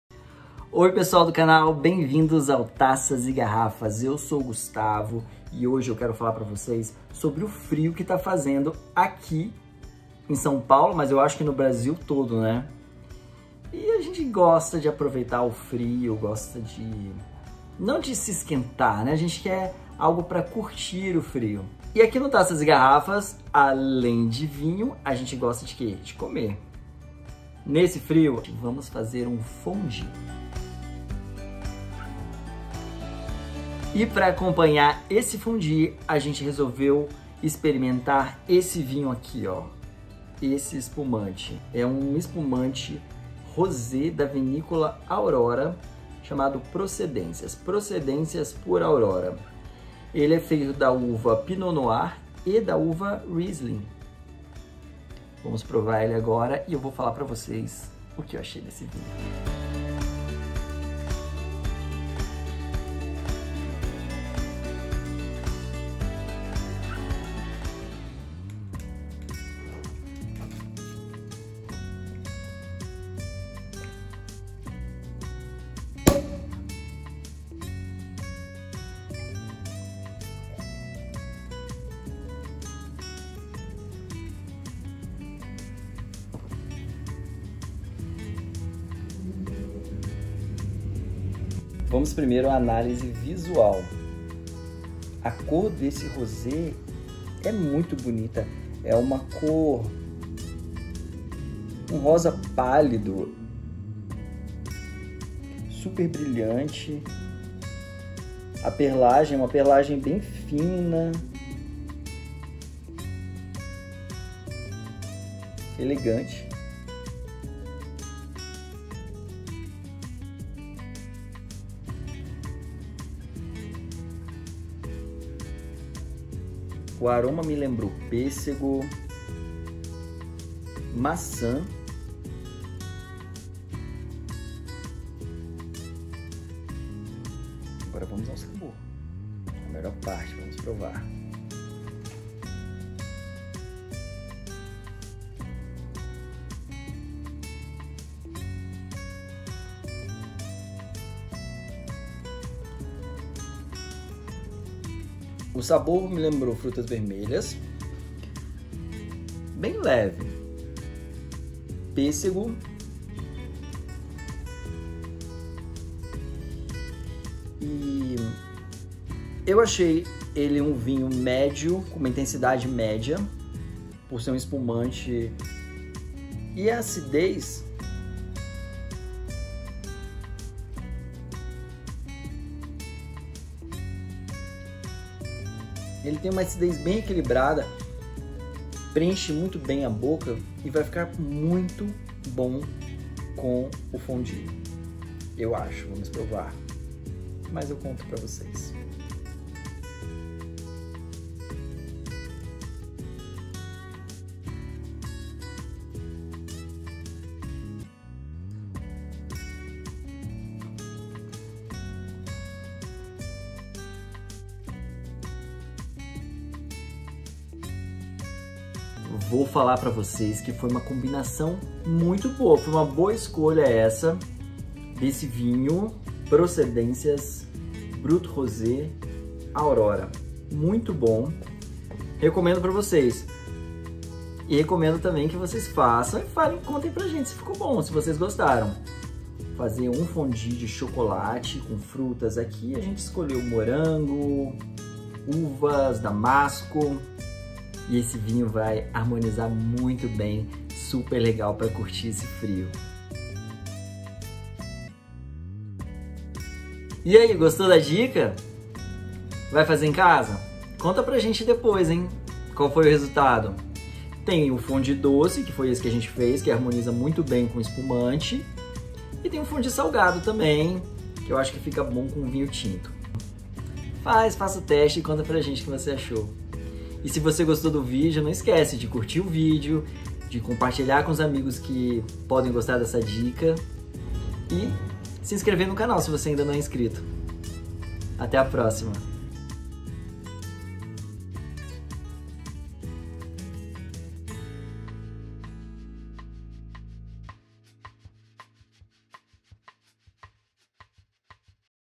DEGUSTAÇÃO: Aurora Procedências Rosé Brut
Hoje o Taças e Garrafas vai fazer a degustação dom espumante brasileiro delicado, de perlagem fina e elegante. Elaborado à partir das uvas Pinot Noir e Riesling, traz sabores frutados leves que remetem ao pêssego e morango. Tudo isso contido num líquido de coloração em tom rosa pastel.

A Serra Gaúcha fica no estado do Rio Grande do Sul. Ela está na zona do clima temperado oceânico, que se caracteriza por invernos moderadamente frios e verões amenos.

A famosa região do Vale dos Vinhedos, bem como as famosas cidades de Gramado, Canela, Caxias do Sul e Bento Gonçalves, entre outras, se localizam na região da Serra Gaúcha.